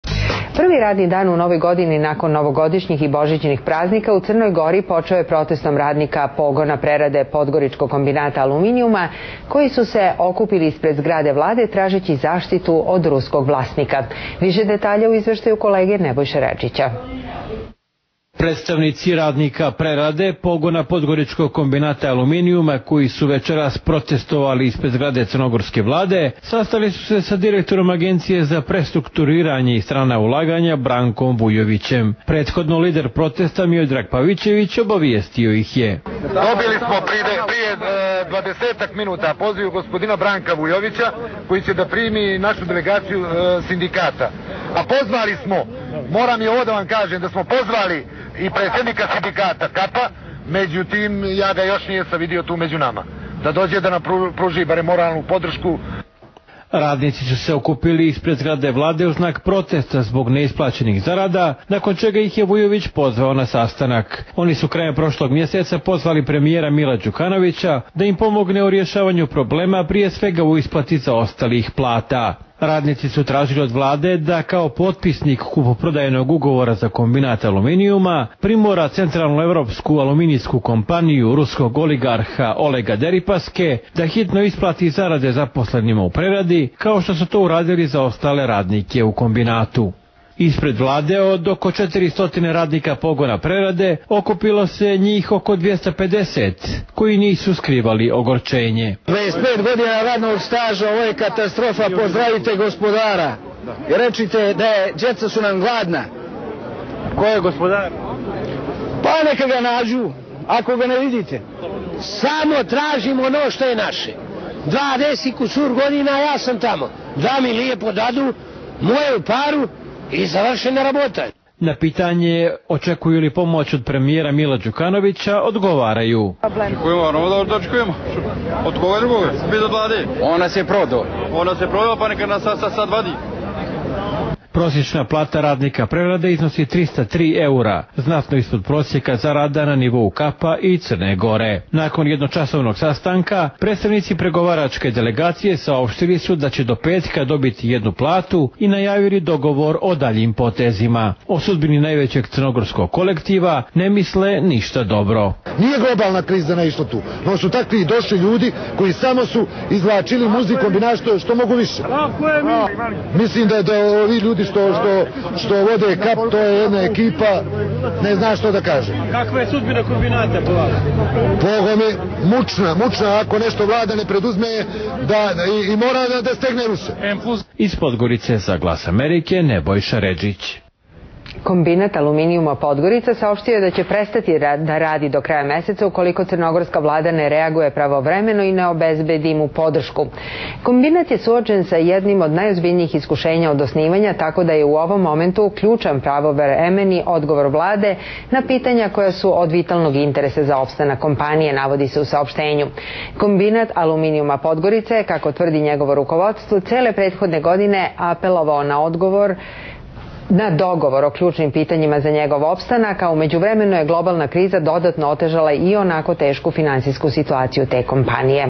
Štrajk pogona prerade u Kombinatu aluminijuma u Podgorici
Prvi radni dan u novoj godini nakon novogodišnjih i božićnih praznika, u Crnoj Gori je počeo protestom radnika pogona prerade podgoričkog Kombinata aluminujuma koji su se okupili ispred zgrade Vlade tražechi zaštitu od ruskog vlasnika.